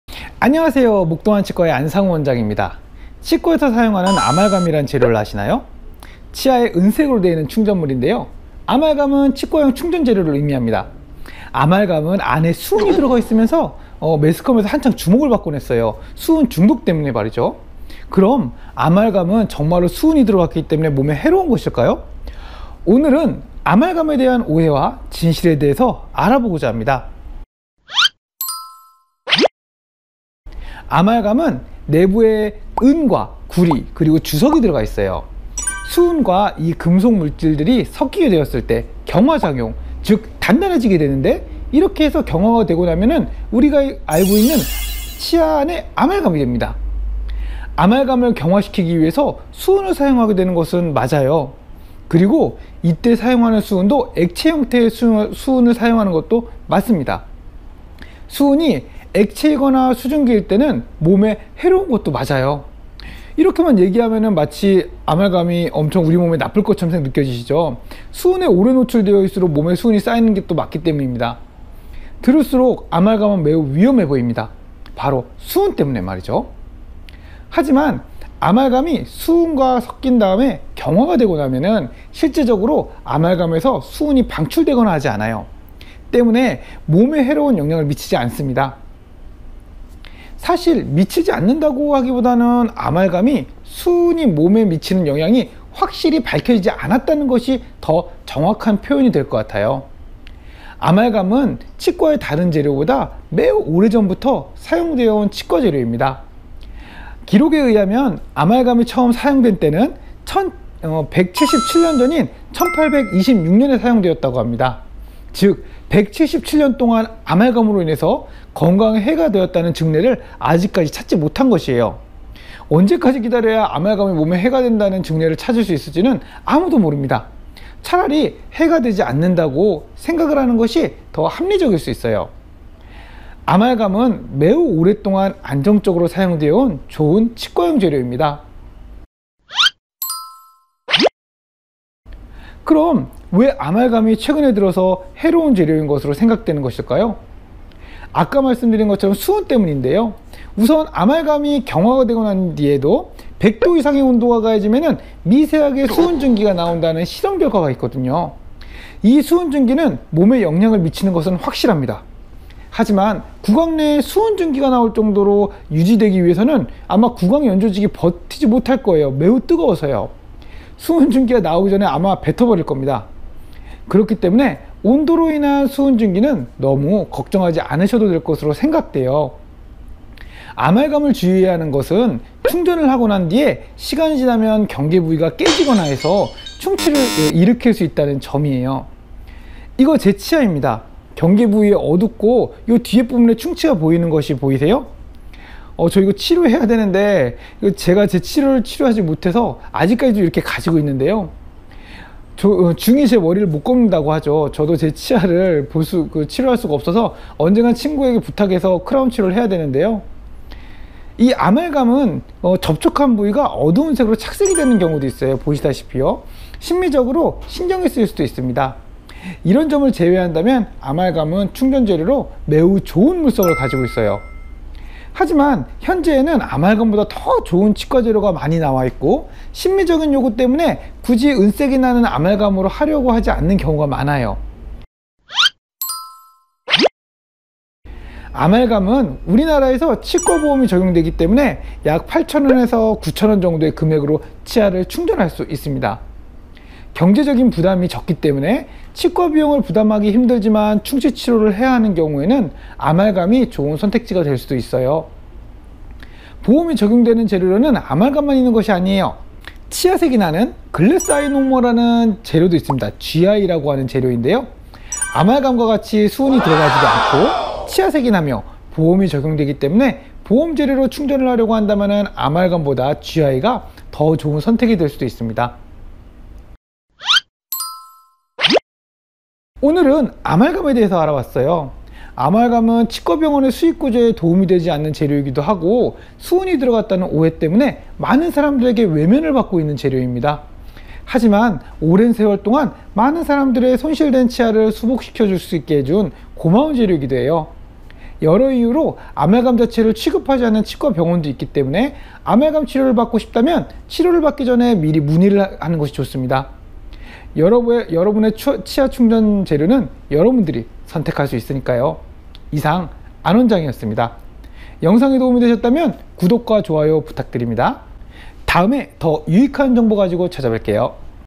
아말감이라는 치과재료에 대한 고찰(029)
아말감은 매우 오랬동안 치과에서 사용되어 온 치과용 재료입니다.
재료를 경화시킬  때 수은을 사용하게 되는데 이 이 수은이 몸에 어떤 영향을 미칠지에 대해서 알아보고자 합니다 .